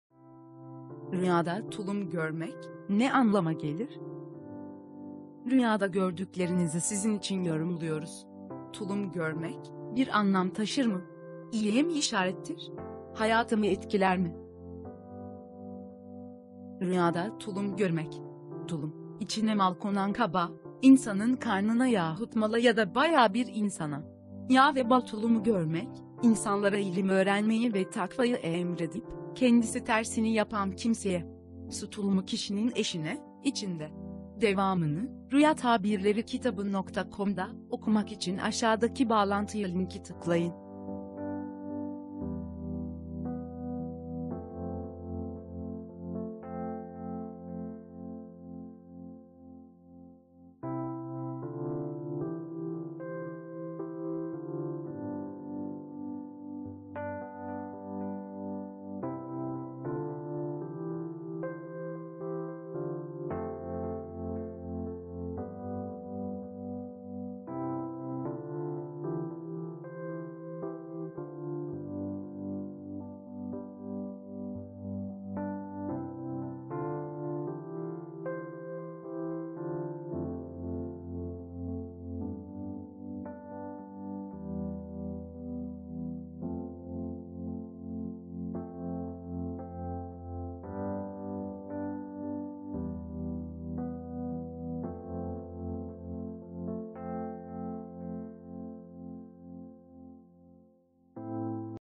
Rüyada Tulum Görmek Ne Anlama Gelir, Ne demektir?
Rüyada Tulum Görmenin Anlamı Nedir? Öğrenmek için
 Tulum
 İçine mal konan kaba, insanın karnına yahut mala ya da bayağı bir insana. Yağ ve bal tulumu görmek, insanlara ilim öğrenmeyi ve takvayı emredip, kendisi tersini yapan kimseye. Su tulumu kişinin eşine, içinde